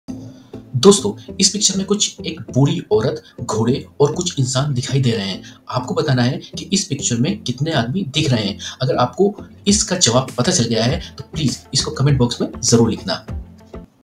Kitne horse ha🤔?
Dosto is picture me kuch ek boodhi aurat ghoodhe aur kuch inssan dikhai de rahe hai
apko batana hai ke is picture me kitne Goodhe dikh rahe hai. agar apko is ka jawab pata chall gaya hai
toh please isko comment box me zarrror likhna